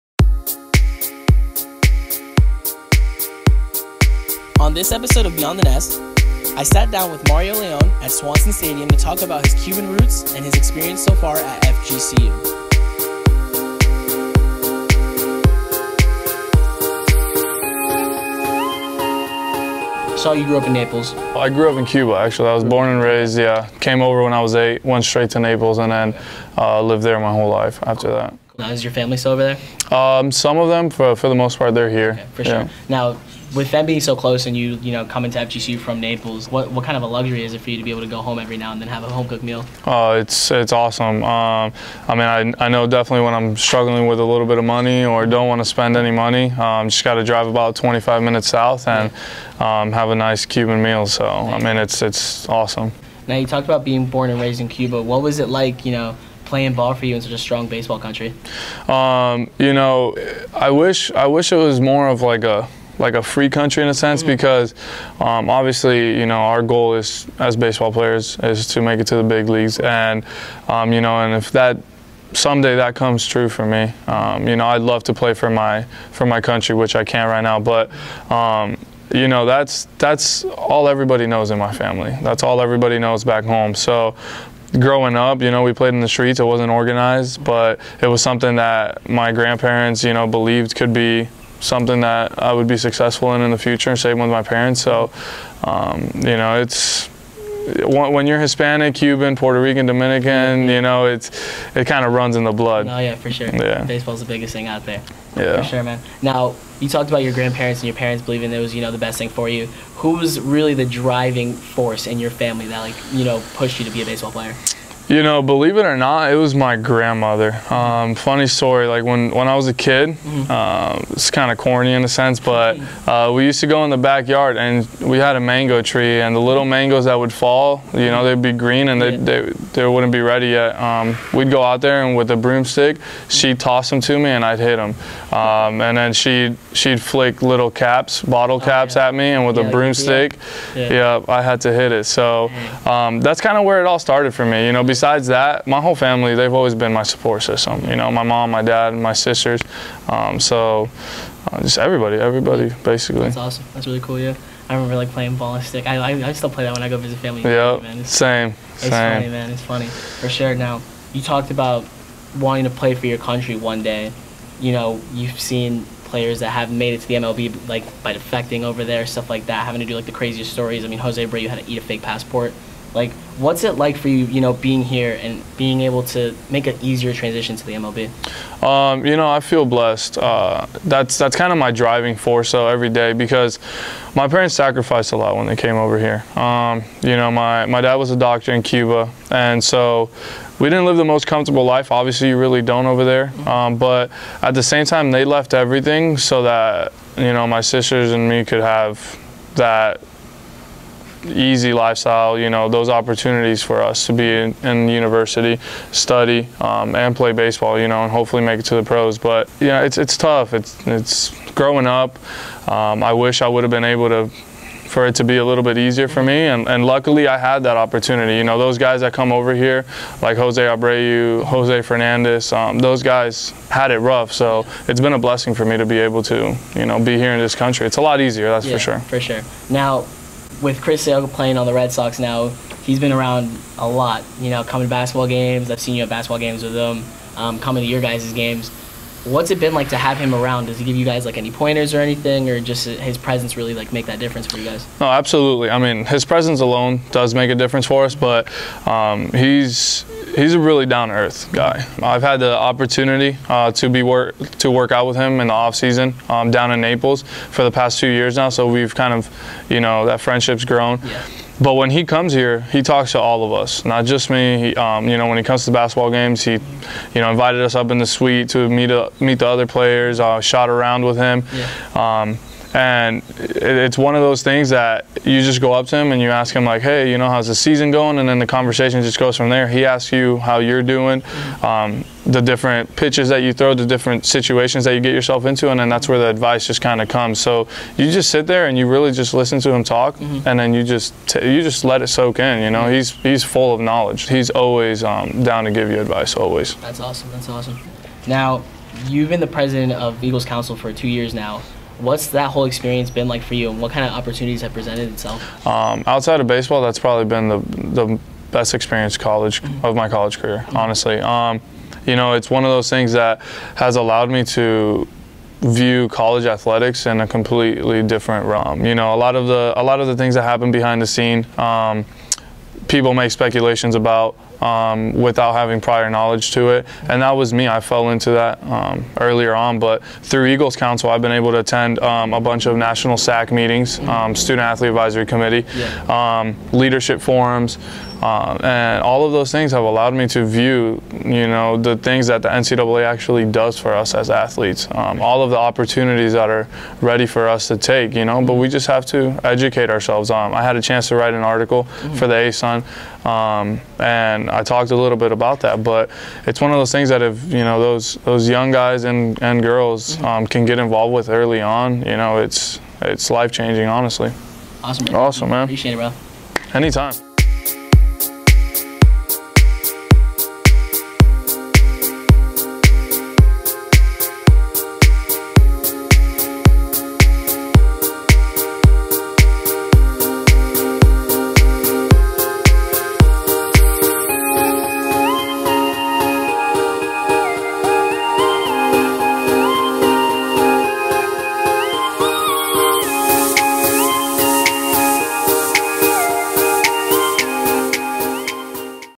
Beyond the Nest: Ep. 4 - Mario Leon
Eagle News Sports Editor Miguel Rodriguez sits down with FGCU baseball's Mario Leon at Swanson Stadium to talk about being raised in Cuba and his experience so far at FGCU in the fourth episode of Beyond the Nest.

For more on Mario Leon and FGCU baseball, head to eaglenews.org.

Filmed by Katie Hopkins and Brad Young. Edited by Katie Hopkins.

Graphics by Audrey Mobley.